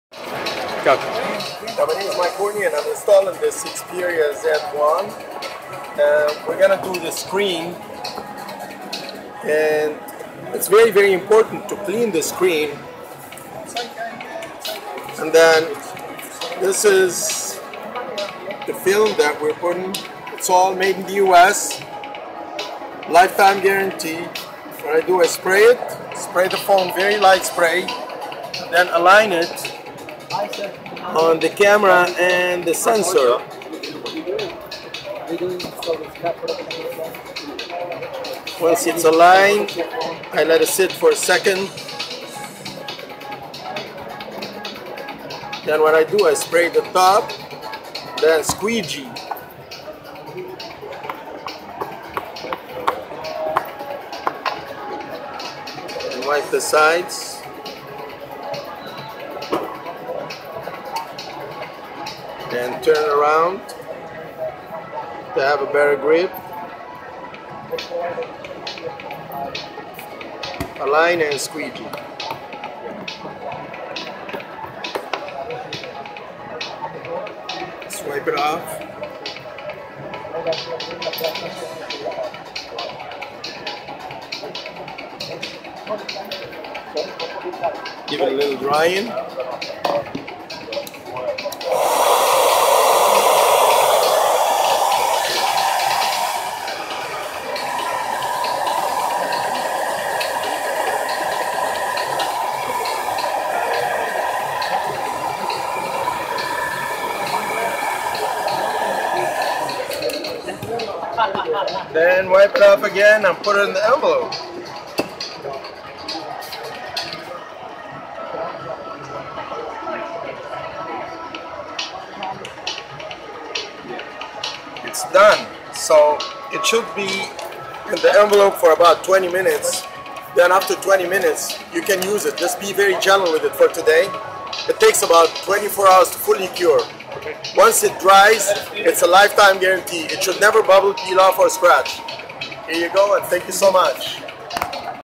Clearcoat UK protector being applied to Xperia Z1
Clearcoat UK screen protector being applied to a Xperia Z1